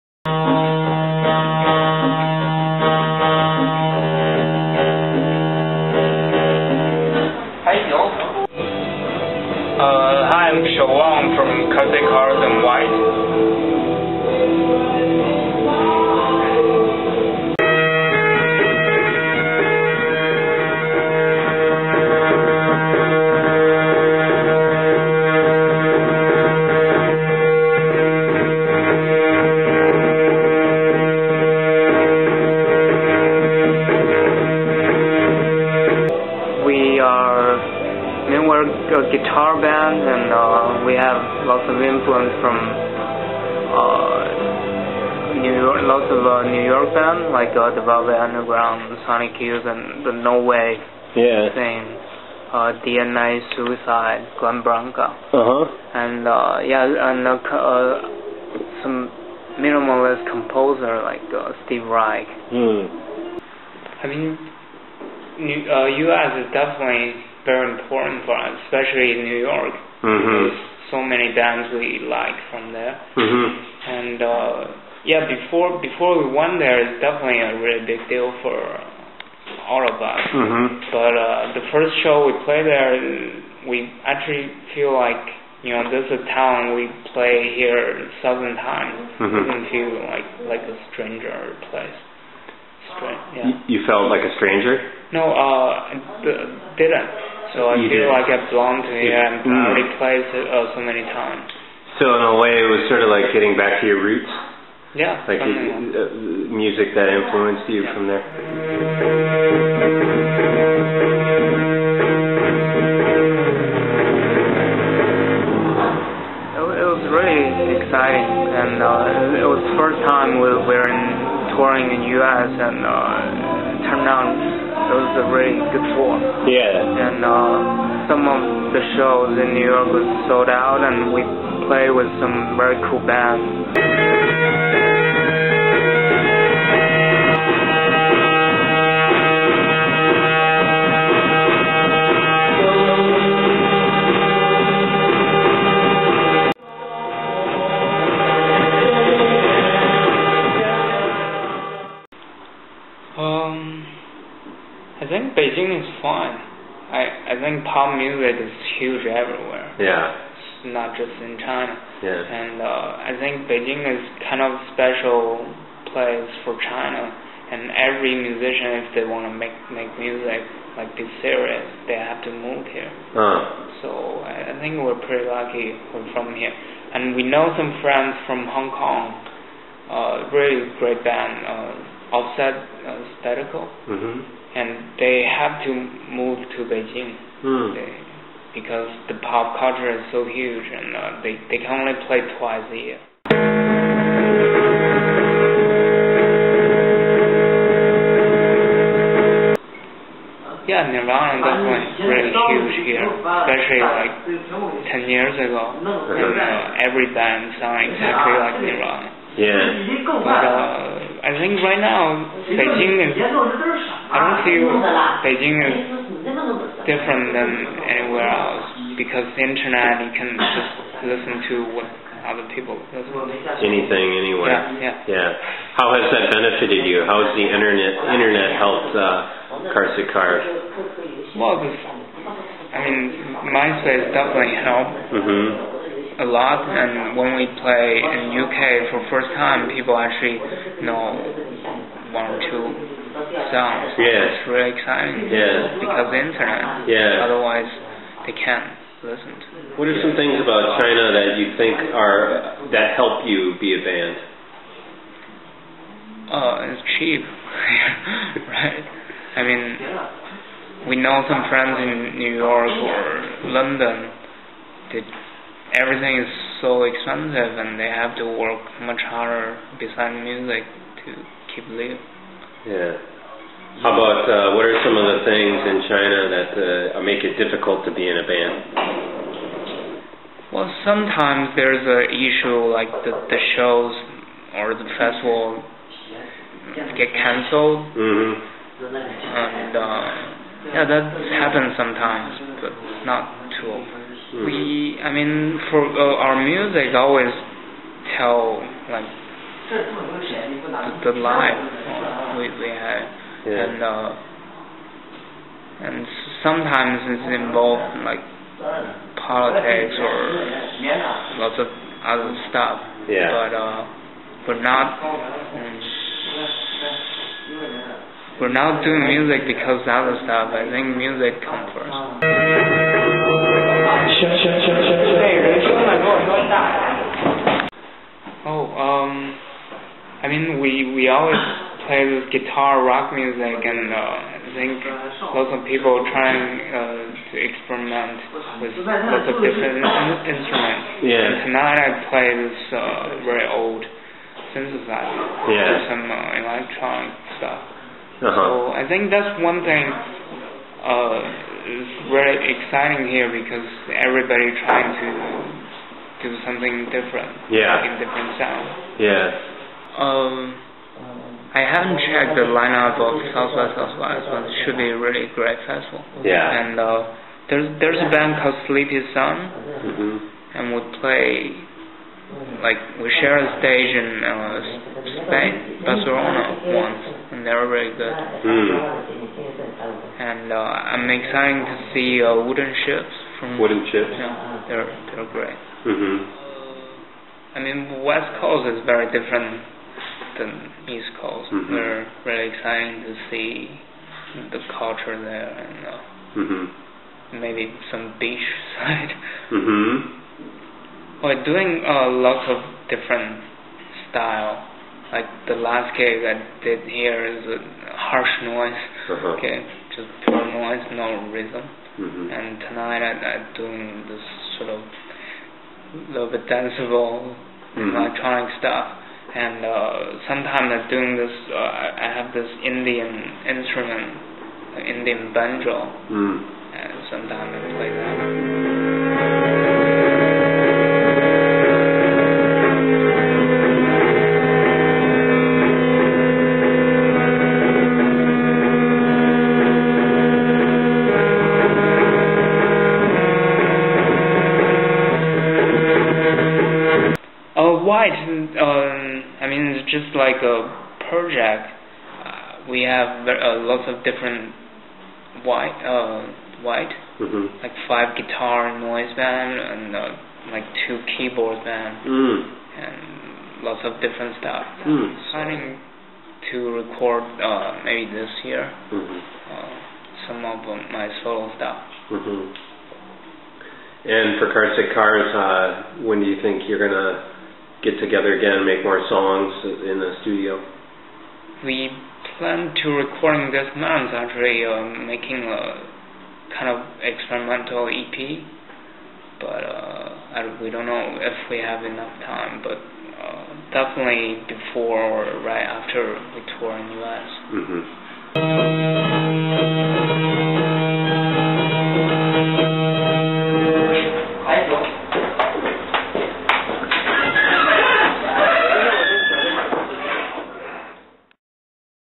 Interview with Zhang Shouwang of Carsick Cars and White at D-22 Beijing, China
Interview with Zhang Shouwang talking about his music, influences and touring the U.S.